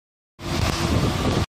Waxahachie Ford Customer Review - Sean M
Sean worked with Johnie Thomas and is the proud new owner of a new Ford F-150!